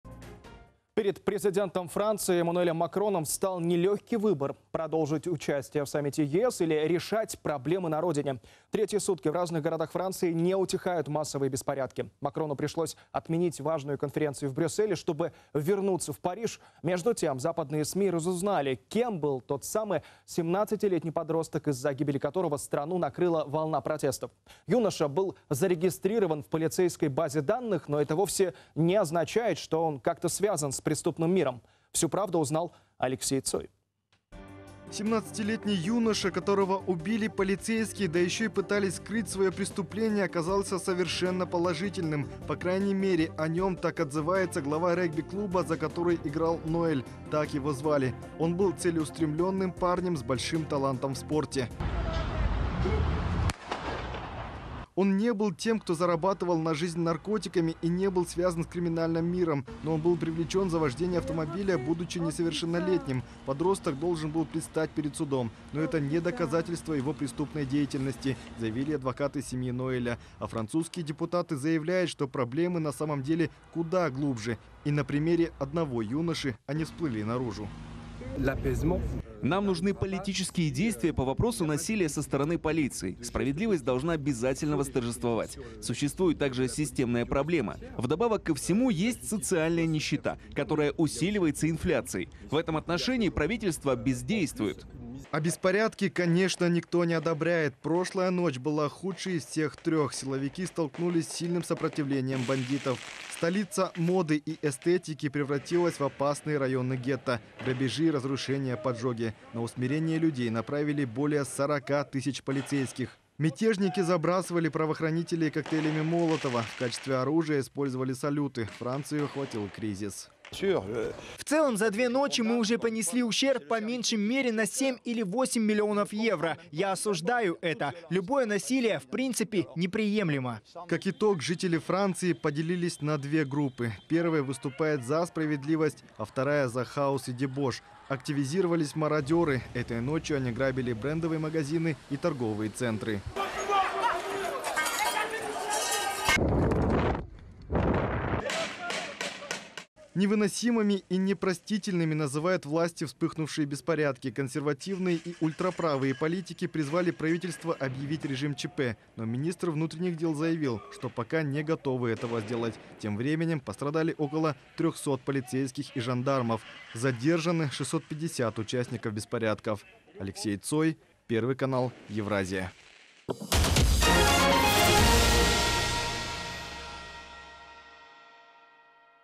КЕМ БЫЛ 17-ЛЕТНИЙ ПОДРОСТОК, КОТОРОГО УБИЛИ ПОЛИЦЕЙСКИЕ ВО ФРАНЦИИ?
Перед президентом Франции Эммануэлем Макроном встал нелегкий выбор. Продолжить участие в саммите ЕС или решать проблемы на родине. Третьи сутки в разных городах Франции не утихают массовые беспорядки. Макрону пришлось отменить важную конференцию в Брюсселе, чтобы вернуться в Париж. Между тем, западные СМИ разузнали, кем был тот самый 17-летний подросток, из-за гибели которого страну накрыла волна протестов. Юноша был зарегистрирован в полицейской базе данных, но это вовсе не означает, что он как-то связан с преступным миром.